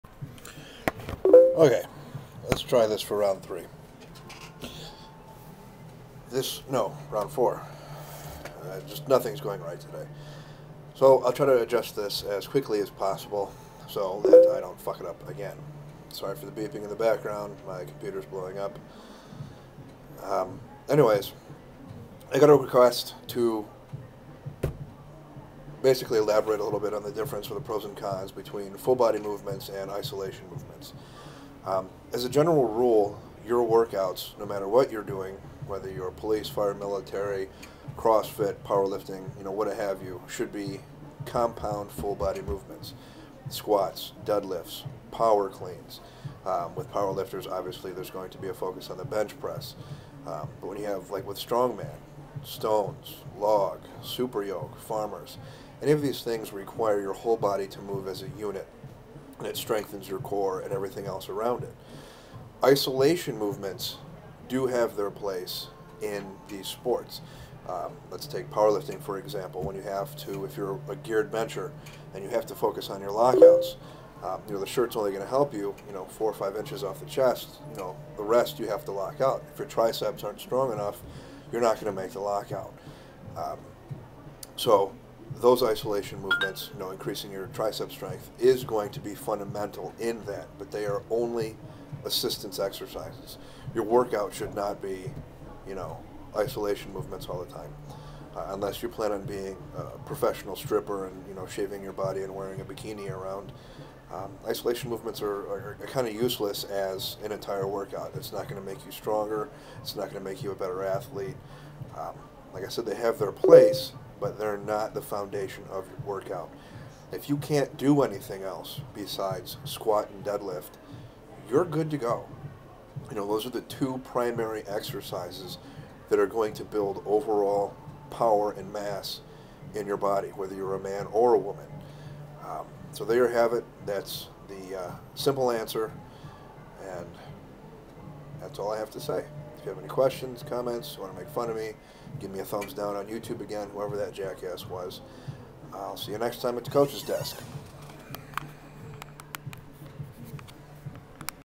From the Coaches Desk-Compound or Isolation movements
Someone asked for my input on whether they should be doing compound or isolation movements so...here it is..